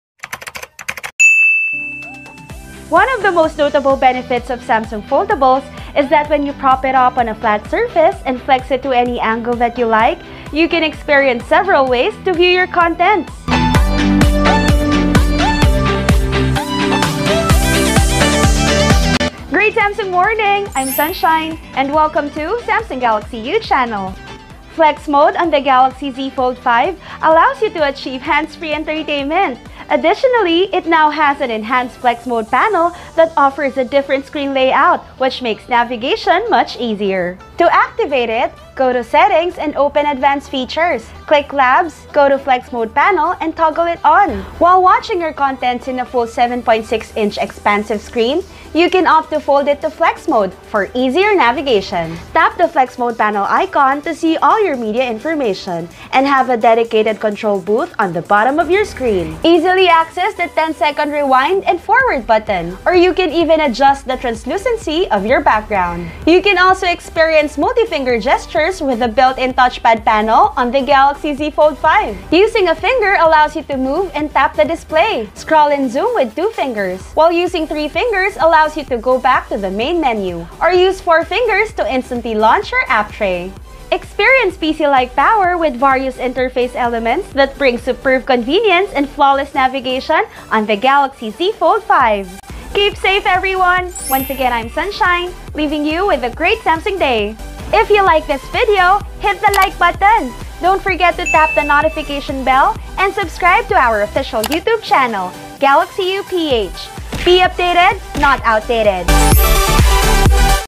Did you know? | Galaxy Z Fold5 ft. Flex Mode Panel
One of the most notable benefits of Samsung foldables is that when you prop it up on a flat surface and flex it to any angle that you like you can experience several ways to view your contents. Flex Mode on the Galaxy Z Fold5 allows you to achieve hands-free entertainment! Additionally, it now has an enhanced flex mode panel that offers a different screen layout which makes navigation much easier.